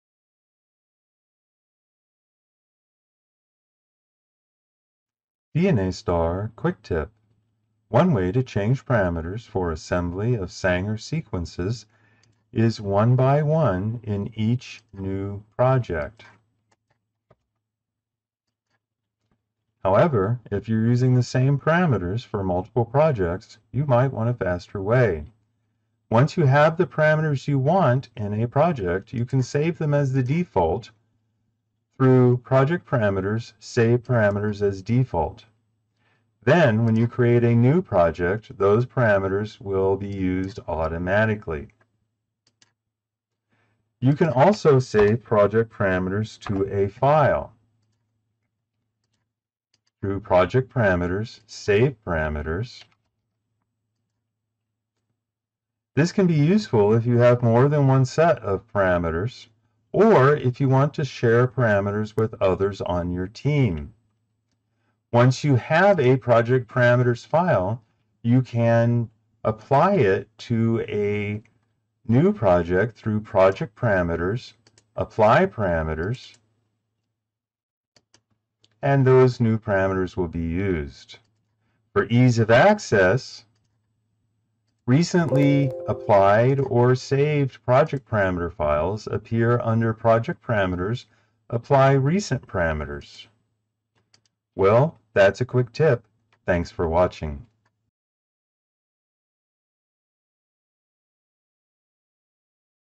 DNASTAR - Saving and applying parameters in SeqMan Ultra
This quick tip video from DNASTAR Senior Software Engineer Andy Thomas shows some easy ways to save and apply custom parameters to your SeqMan Ultra assembly and analysis projects.